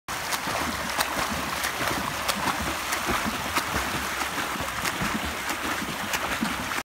Seb Garry hitting a Mad on his swim session in Mallorca 2016
Here we have Seb Garry Double European AG Champion trying out his Sailfish wetsuit in a main swim set in Mallorca